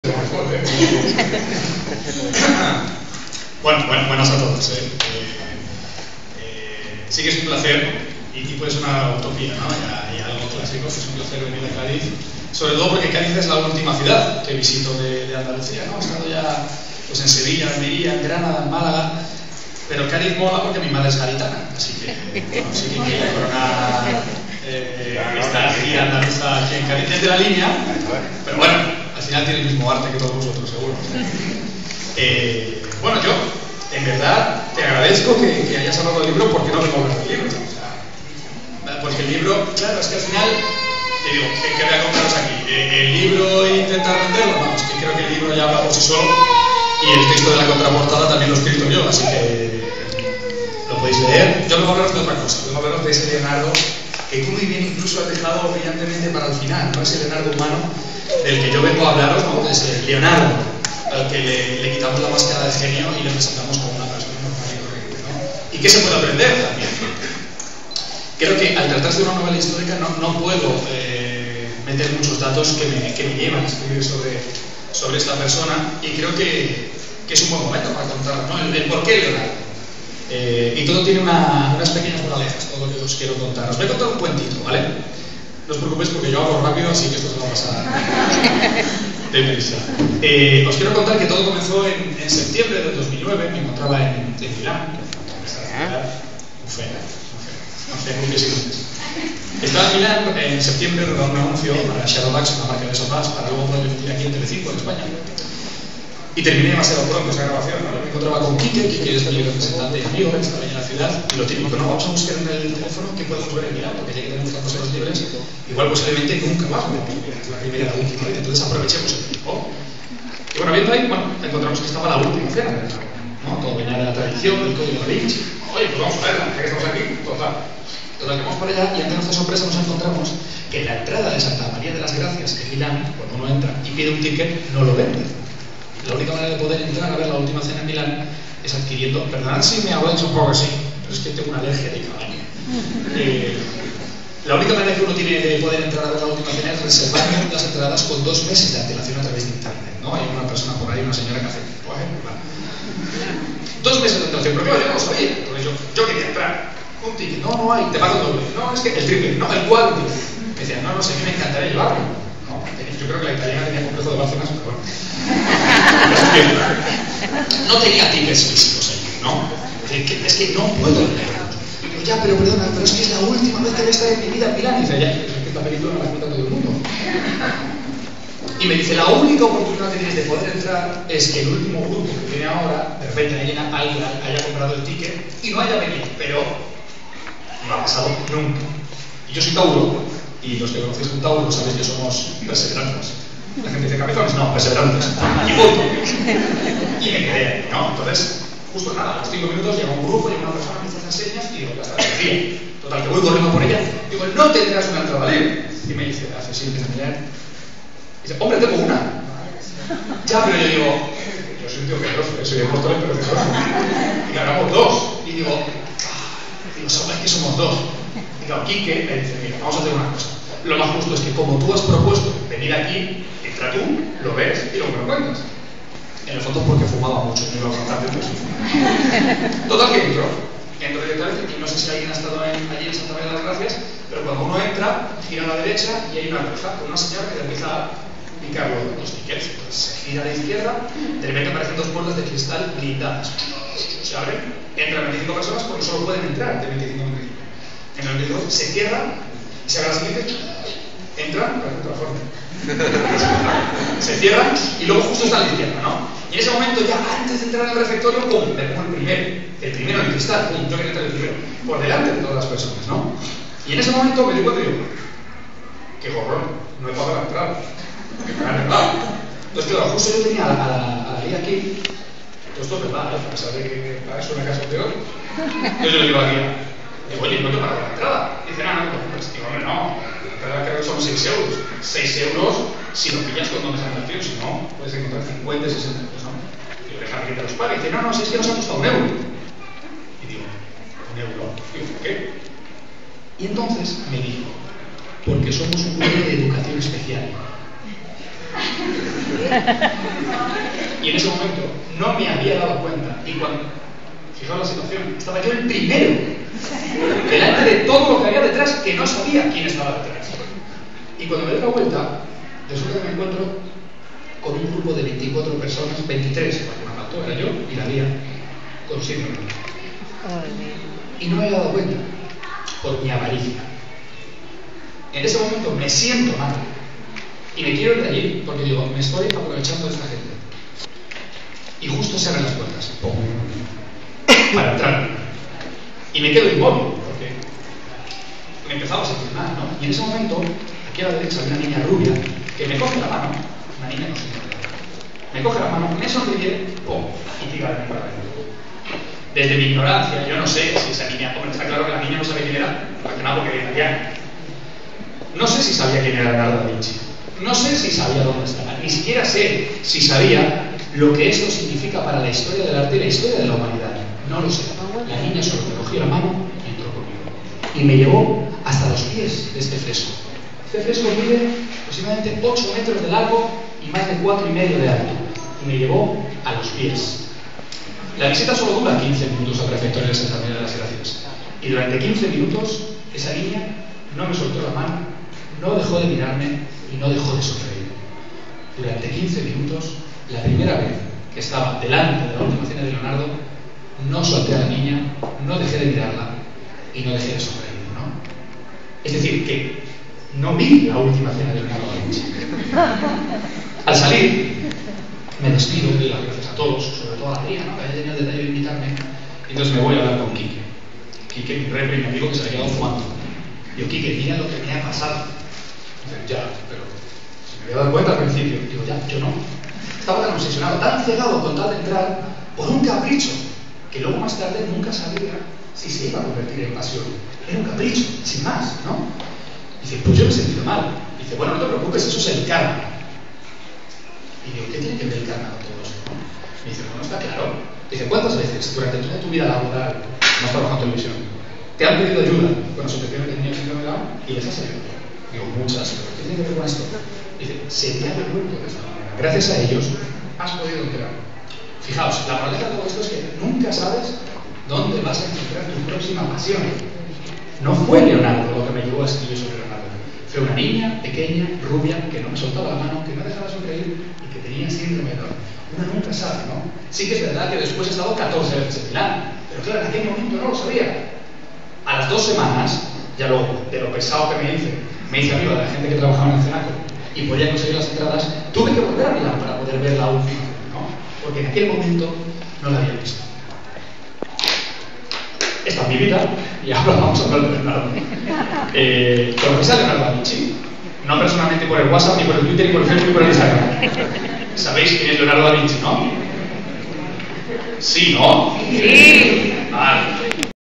Christian Gálvez-Conferencia-
Christian Galvez cuenta una anécdota en la presentación de su libro, sobre Leonardo da Vinci, en Cádiz,